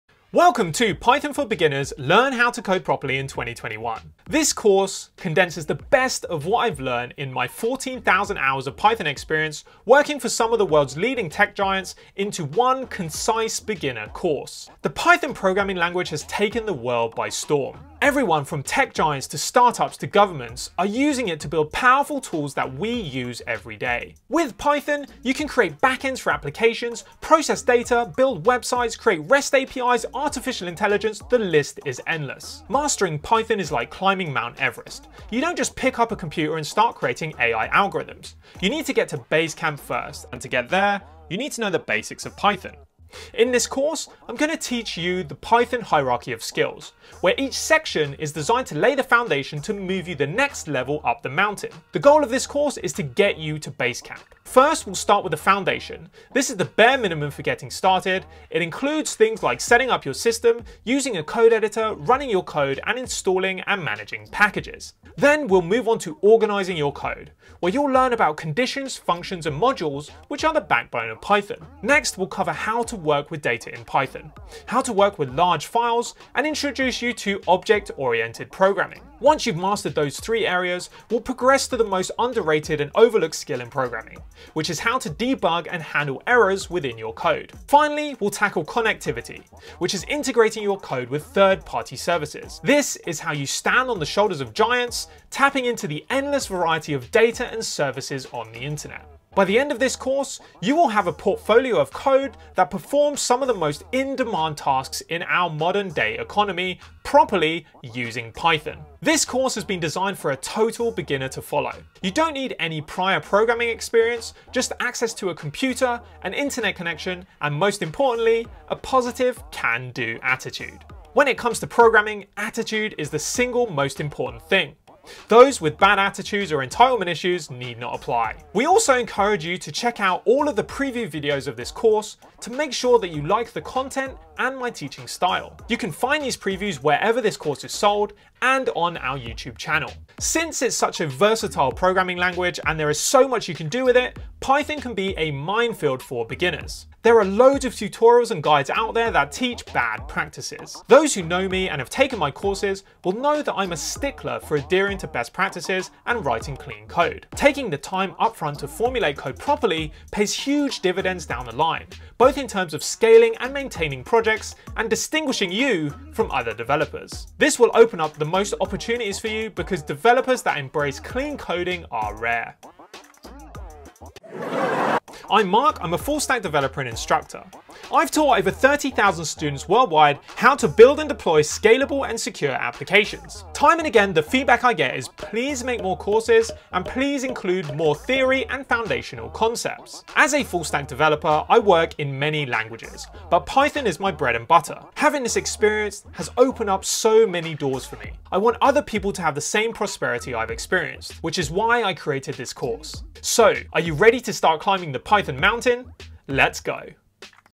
BRAND NEW COURSE: Python for Beginners
This course is designed to teach an absolute beginner the basics of Python.

Python is a language that needs no introduction. It’s incredibly powerful, versatile, fast and it’s easy to learn.

However, given its rapid rise in popularity over the last few years, there are loads of tutorials and guides out there which teach bad practices.

Our goal is to change that, by creating a course which gives you a head start, by teaching best practices and underlying concepts.

You can preview the first half of the course for free on Udemy: